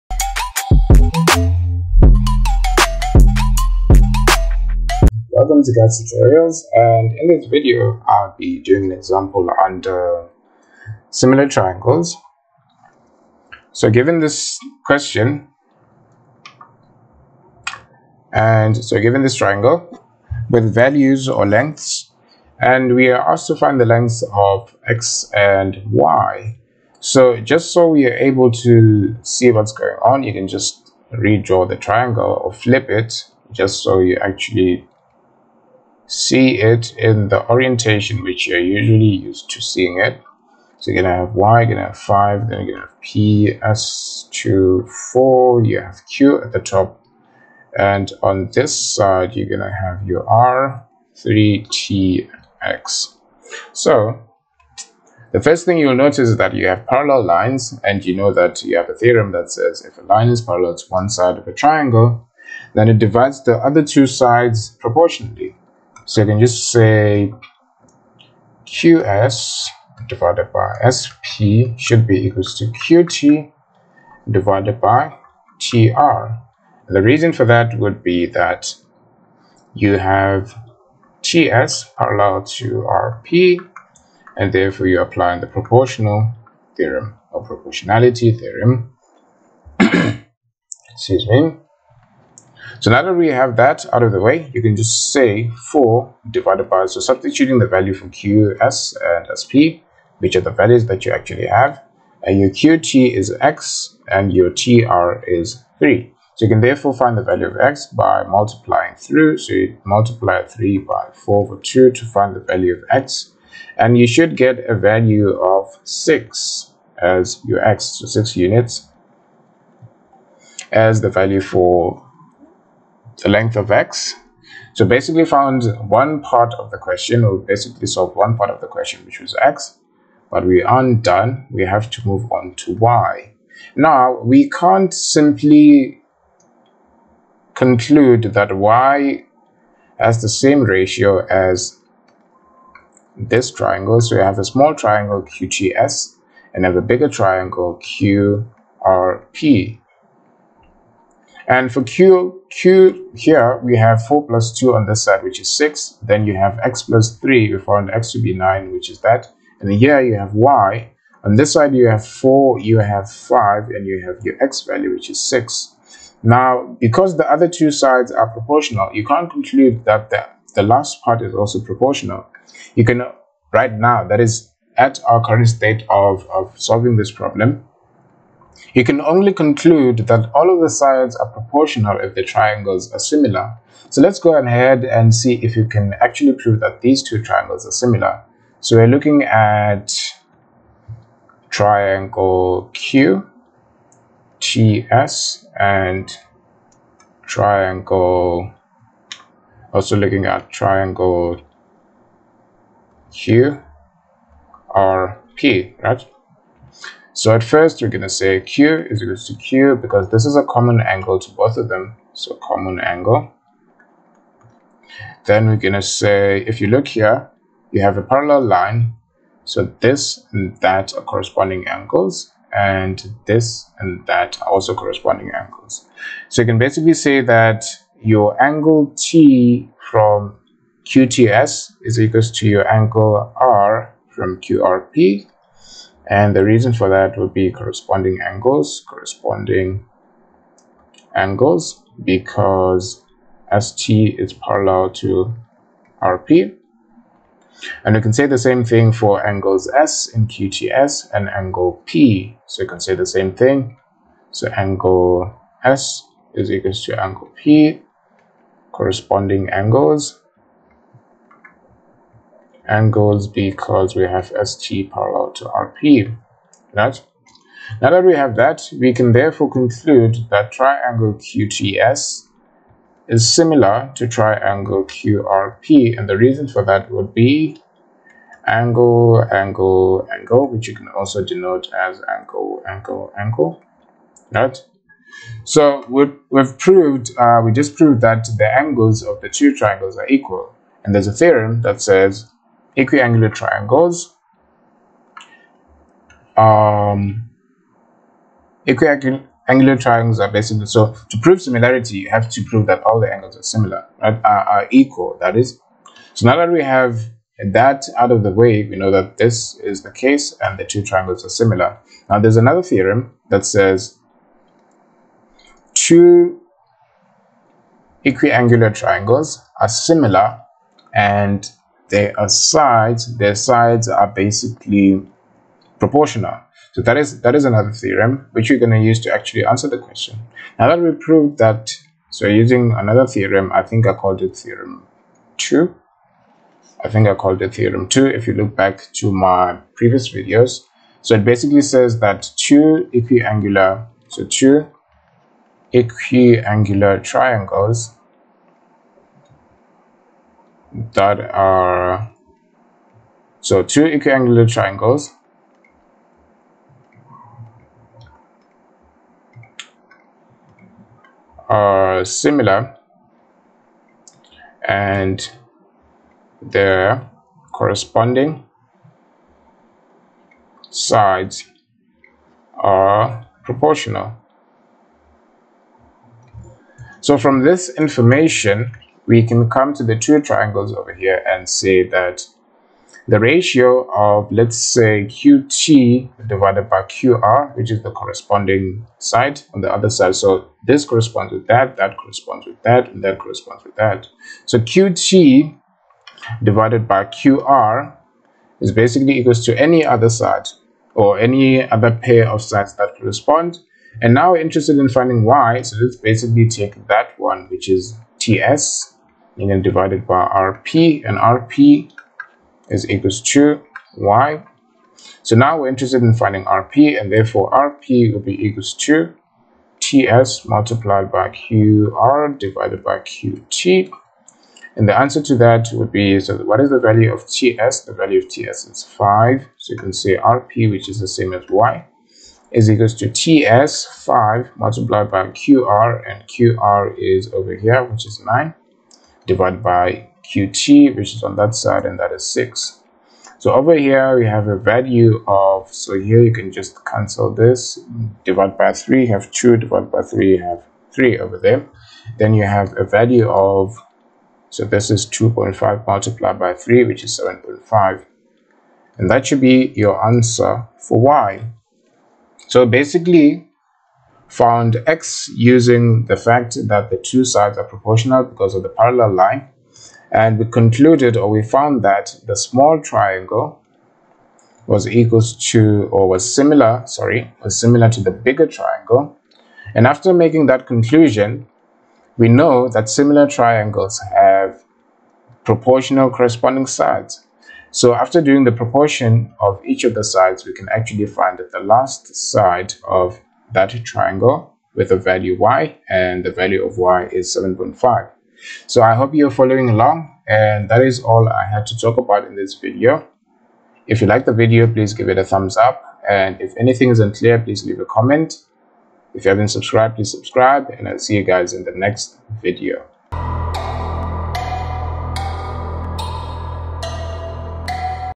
Euclidean Geometry: Similarity Example | Grade 12 Mathematics | Khetz Tutorials
Showing how to use the proportionality theorem and similarity to solve problems.